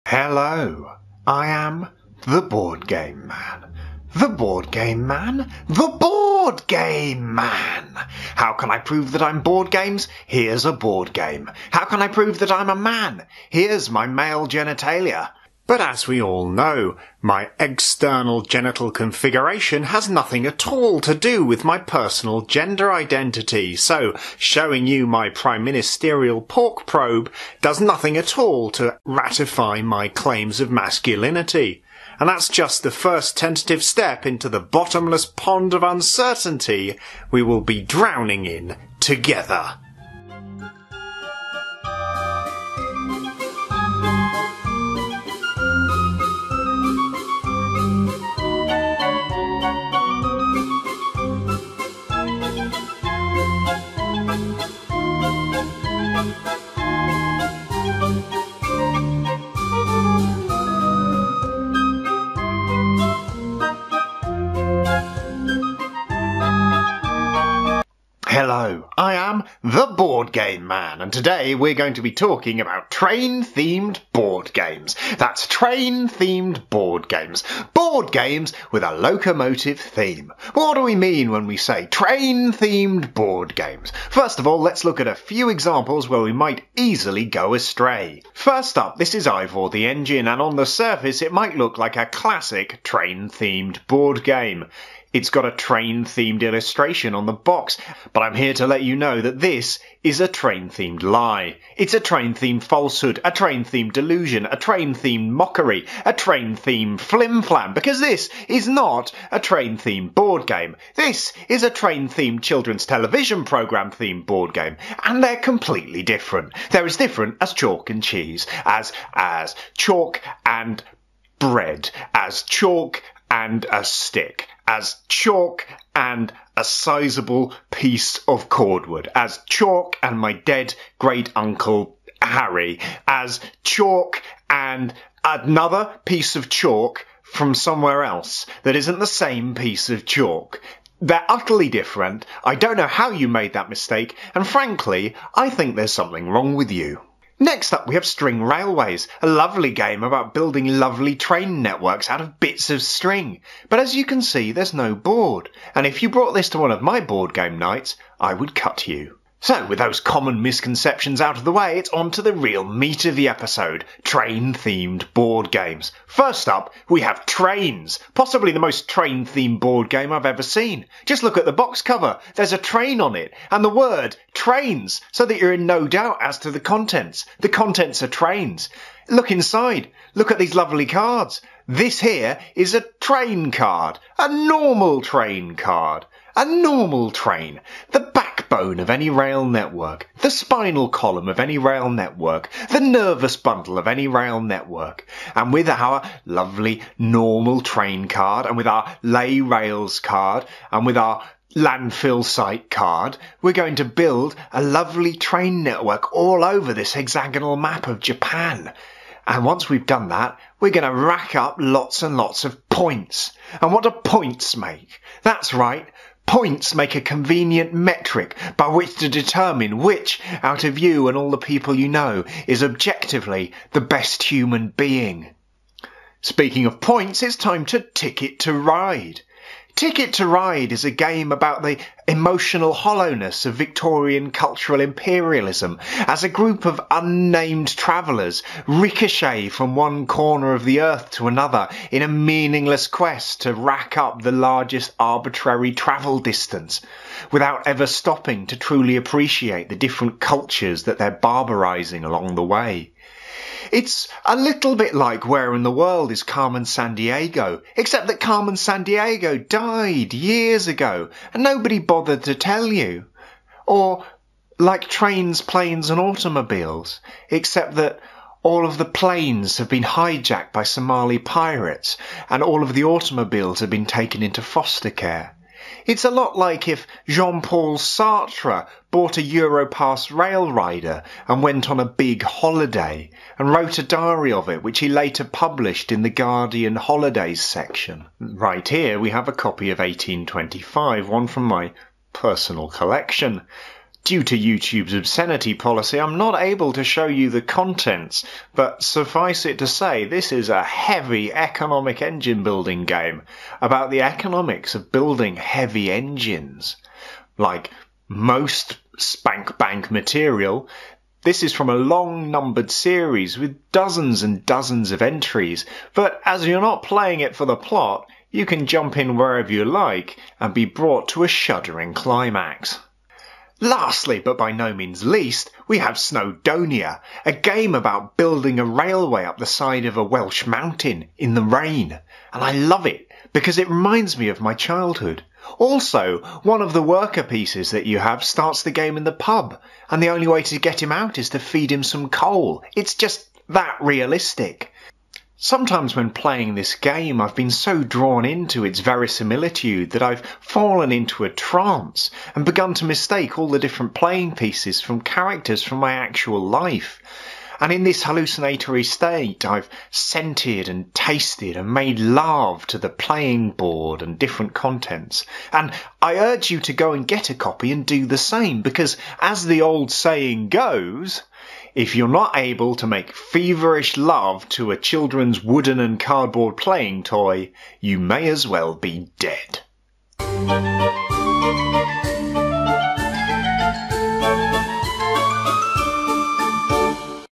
The Board Game Man - Train Themed Boardgames
Are you Board Games? Do you tabletop? Do you remember the smell of rain from the places of your childhood? Your school playground perhaps? Or the waterfront by your grandparents' house? Do you want to learn about Train Themed Boardgames? Then this is your Board Game Man!